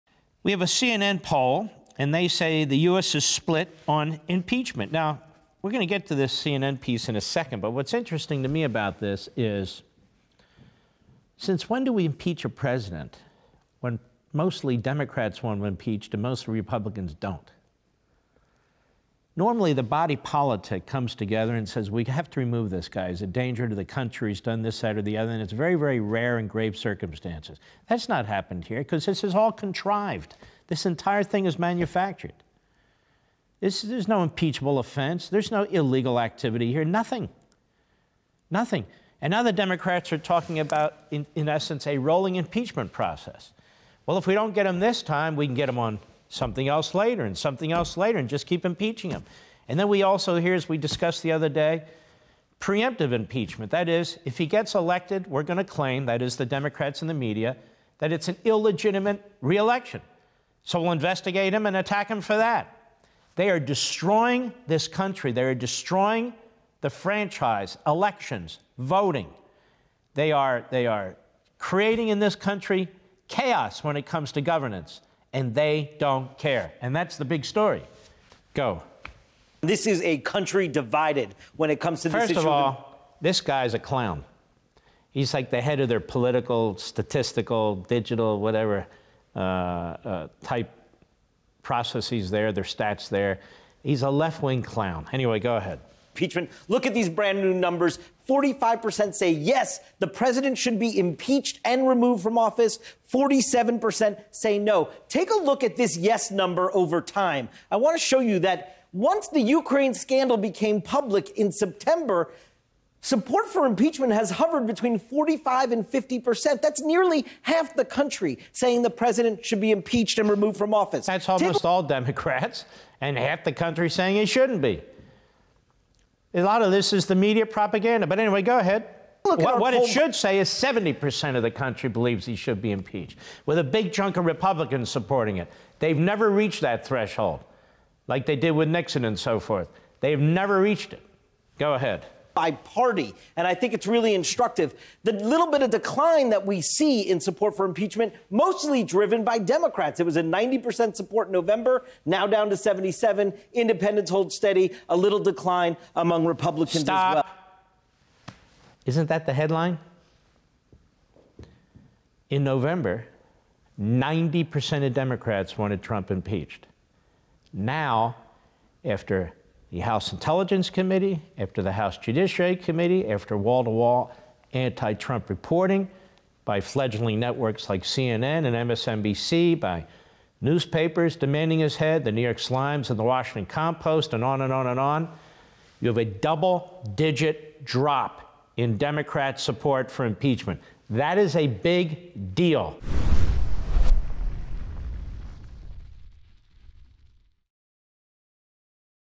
Mark Levin: CNN Can't Believe It's Own Poll on Impeachment
Mark Levin reacts to CNN's latest blunder on impeachment.